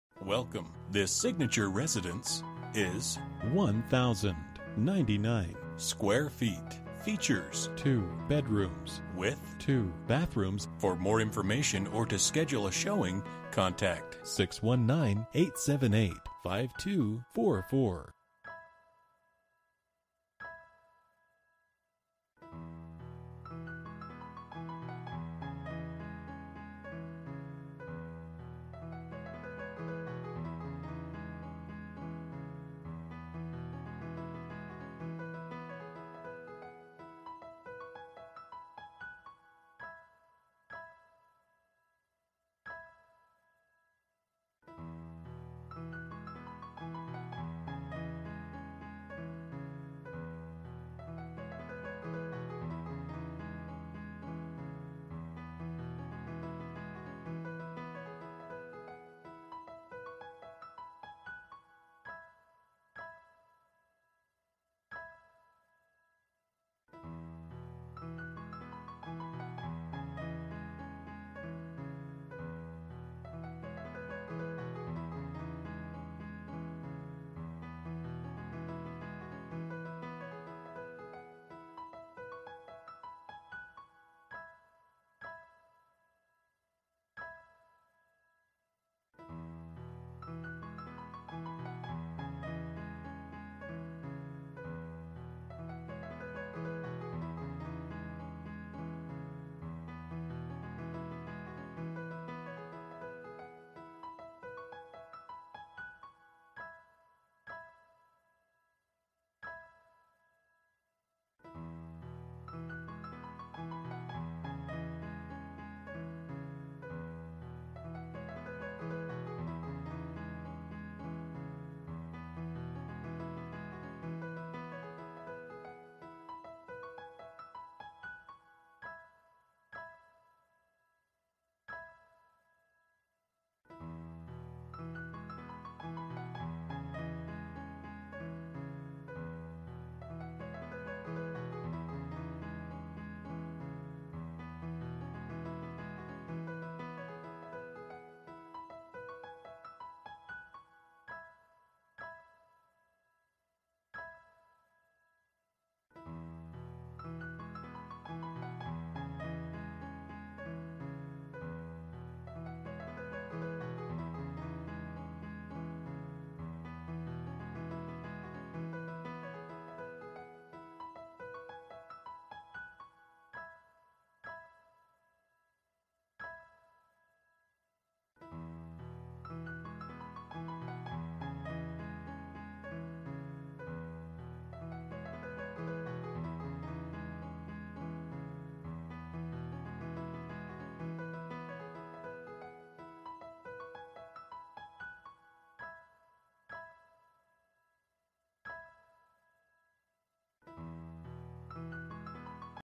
Residential for sale - 1810 Avenida Del Mundo 709, Coronado, CA 92118
- Sweeping views of the Pacific Ocean to Mexico and Coronado Bay Bridge! This 2BR split floor plan is move-in ready and features terrazzo tile floors, modern white vinyl cabinetry with granite counters and back-splash, GE profile SS appliances, custom lighting in the dining w/ LED dimmers, and custom built-in entertainment center and shelving in the living room. Two decks provide over 130sf of outdoor living space. Newer ClimateMaster w/ Nest thermostat for energy efficient heat/air

Bedrooms: 2
Bathrooms: 2
Square Feet: 1099
Price: $1,630,000